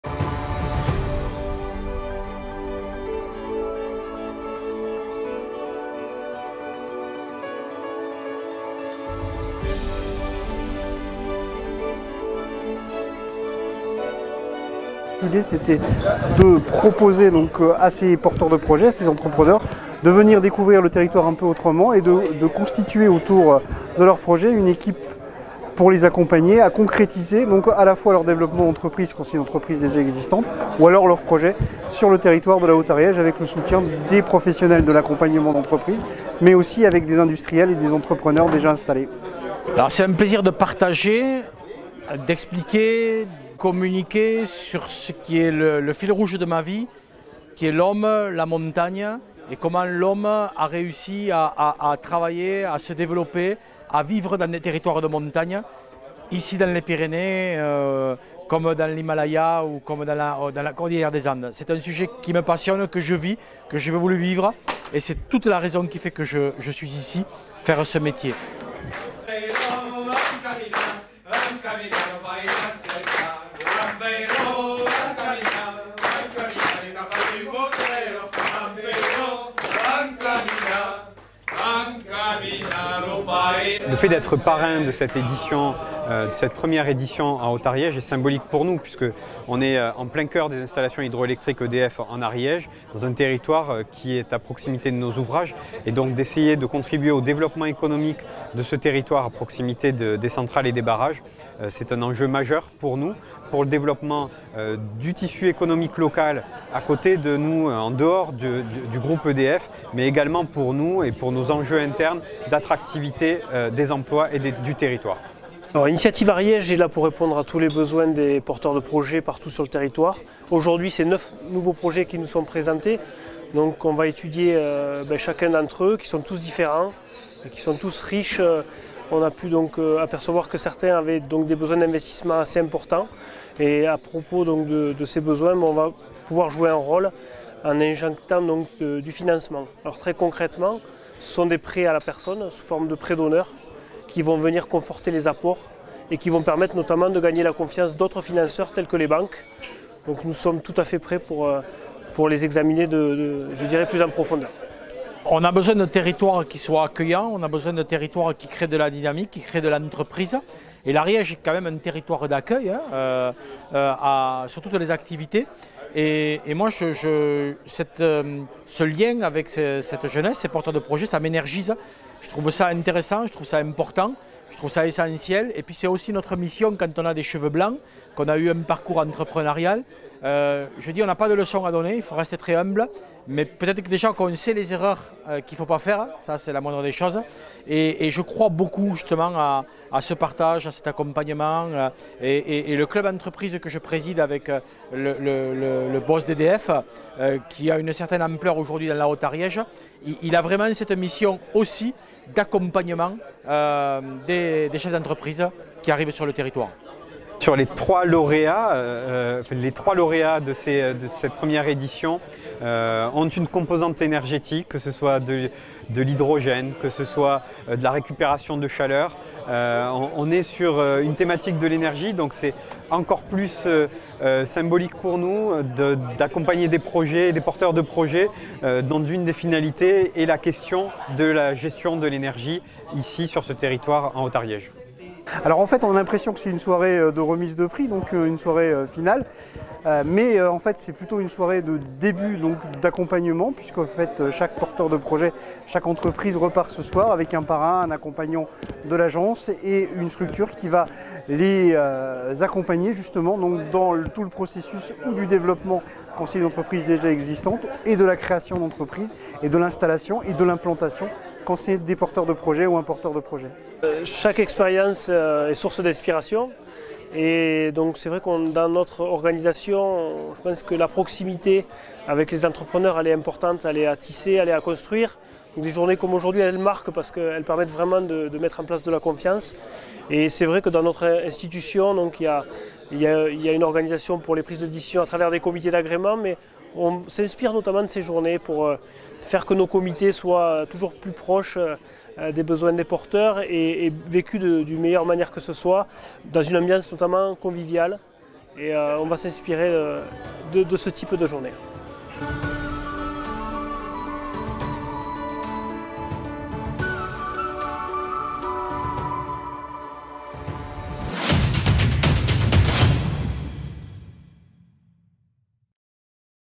Création d'Entreprise :l'Ariège amène des porteurs de projet en Estives
C'était samedi dernier, le 19 juin, sur le plateau de Beille qu'Ariège Attractivité avait décidé d'amener les 9 projets sélectionnés dans le cadre de son opération Les Estives de l'Entrepreneur ", 45 étudiants, porteurs de projets , professionnels de la Com et chefs d'entreprises accompagnaient aux coté de Philippe Lacube aux manettes pour la journée les porteurs de projets ; une opération séduction dans le but d'accompagner et transformer l'essai en 9 futures entreprises installées en Ariège